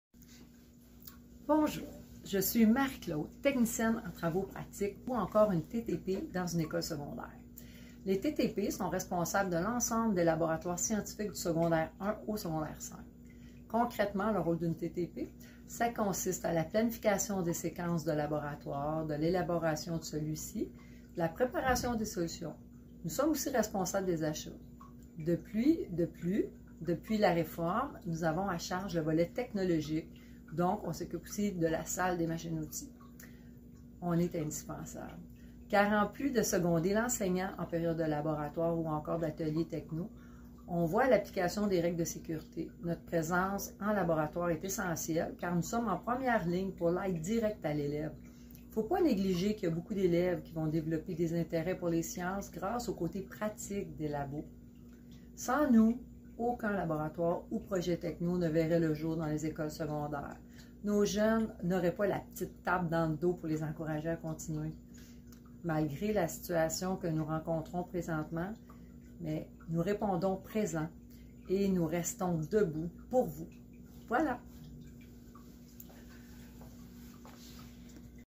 Pourquoi le travail des techniciennes et techniciens en travaux pratiques est essentiel
La FTQ ainsi que ses syndicats affiliés impliqués dans les négociations du secteur public (SCFP-Québec, SQEES-298, SEPB-Québec, UES 800) lancent une vaste campagne de vidéos de témoignages sur les réseaux sociaux pour la valorisation des titres d’emplois des travailleuses et travailleurs de la santé, de l’éducation et des services sociaux.

Du 15 au 26 mars 2021, les travailleurs et travailleuses que nous représentons sont invités à tourner des vidéos dans lesquels on leur demande d’expliquer en quoi consiste leur travail et pourquoi ils sont essentiels au bon fonctionnement des services publics.